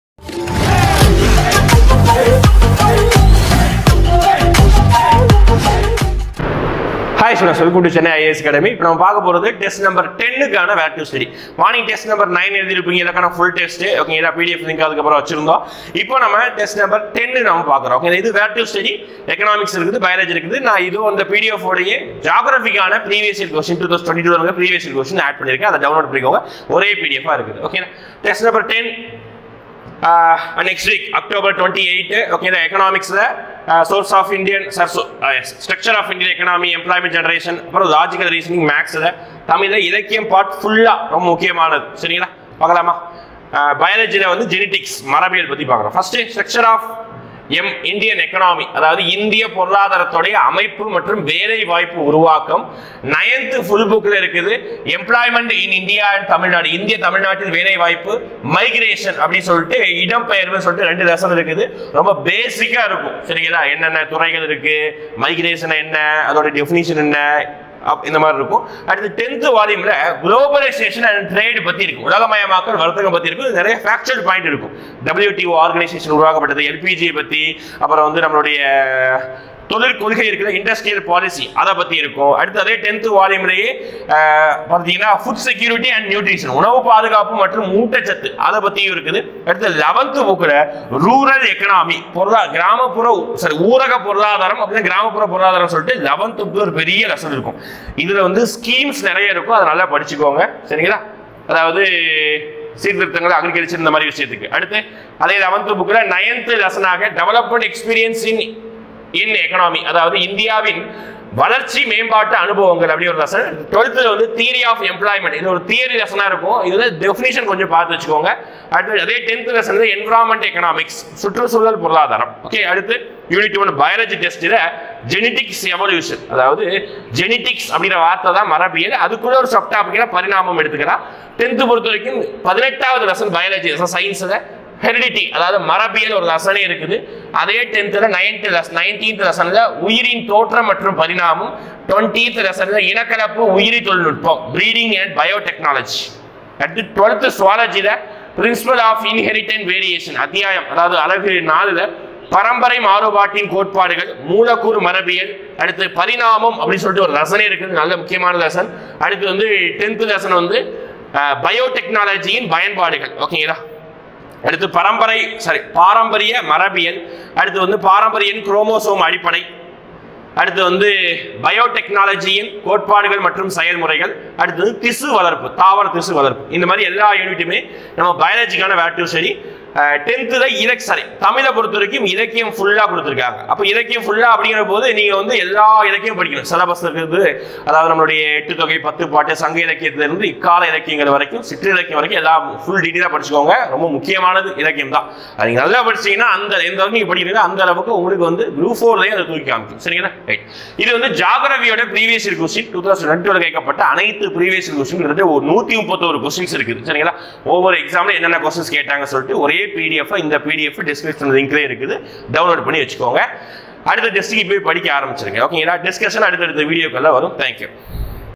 மாற்றம் FREE TEST BATCH | TEST 10 | தமிழ் (இலக்கியம்) பொருளாதாரம், அறிவியல் புவியியல்
TEST 10 | தமிழ் (இலக்கியம்)பொருளாதாரம், அறிவியல் புவியியல் மொத்தமுந்தை ஆண்டு வினாக்கள்  வடிவில்.

** FREE TEST BATCH SCHEDULE **